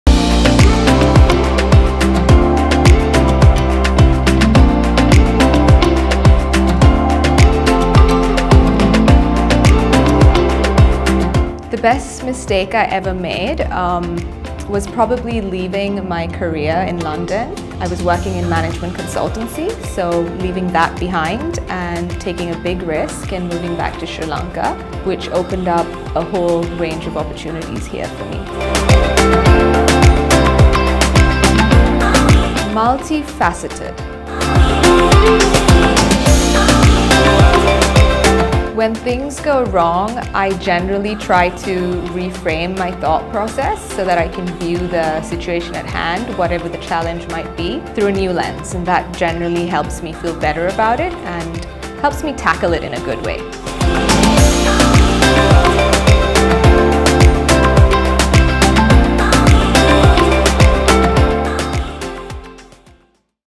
35 under 35 - Niyoshala Kumar - Cosmopolitan Sri Lanka
Niyoshala Kumar, Director at Beverly Street,  describes her career, best mistake and her go-strategy when things go wrong
Presented by Sri Lanka Insurance

capitalmedia.lk/product/cosmopolitan/